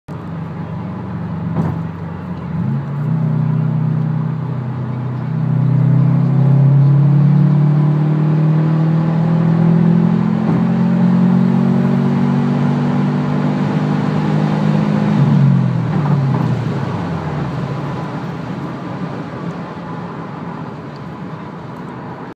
2013 subaru wrx 80-120 highway pull. Stock.
Just a quick 4th gear pull. Invidia n1 race exhaust.